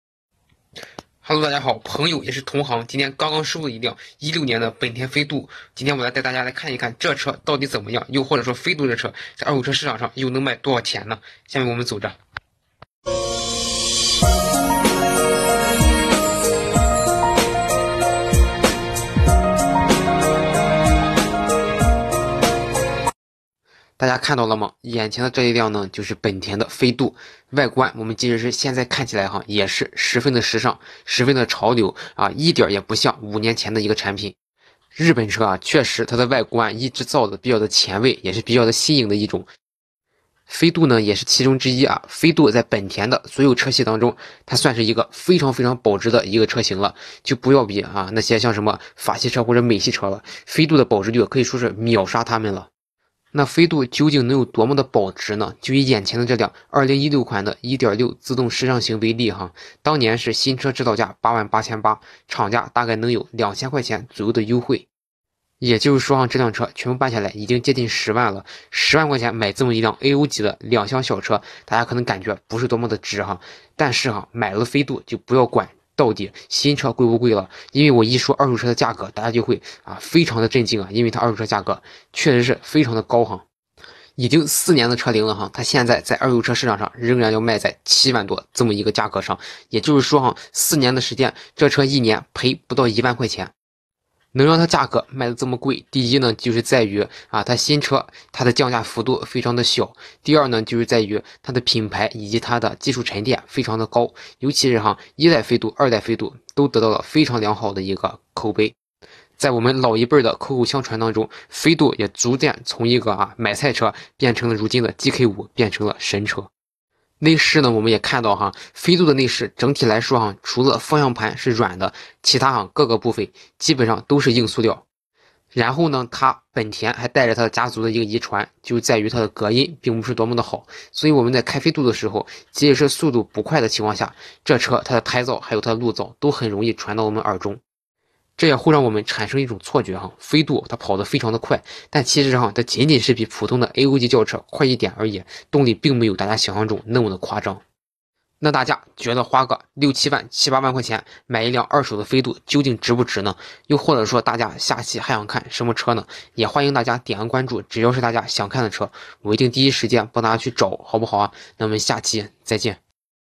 刚收了一辆16年本田飞度，这价格大家觉得怎么样
刚收了一辆16年本田飞度，这价格大家觉得怎么样,本视频由小洋汽车知识原创提供,18022次播放,好看视频是由百度团队打造的集内涵和颜值于一身的专业短视频聚合平台